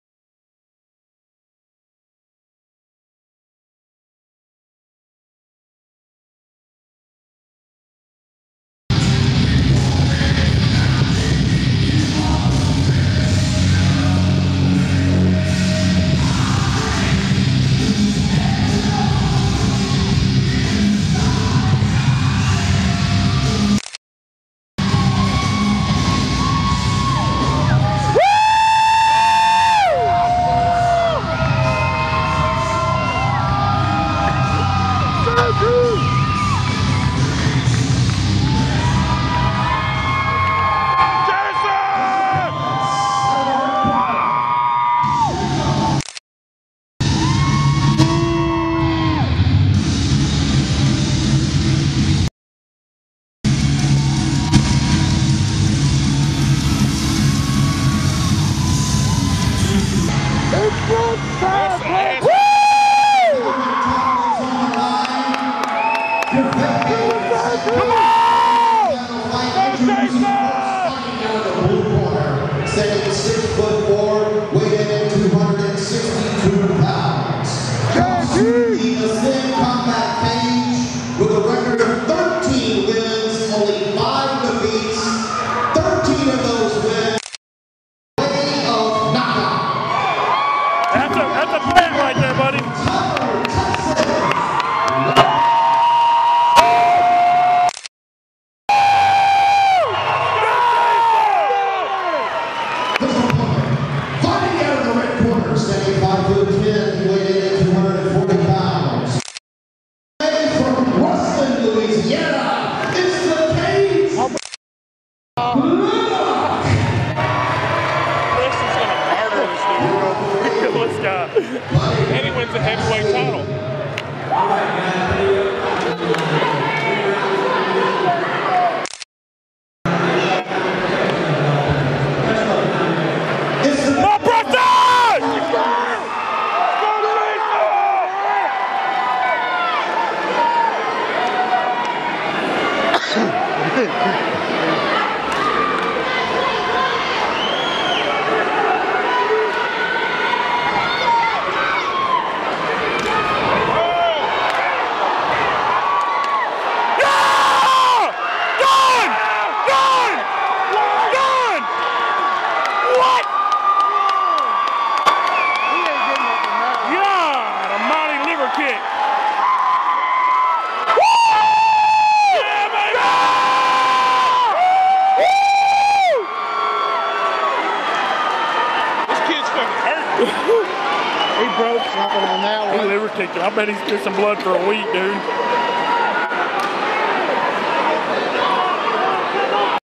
Jason "The Predator" Preston Nothing personal 3
The Predator Takes home The Belt at Ascend Combat's  Nothing Personal 3 in Shreveport, La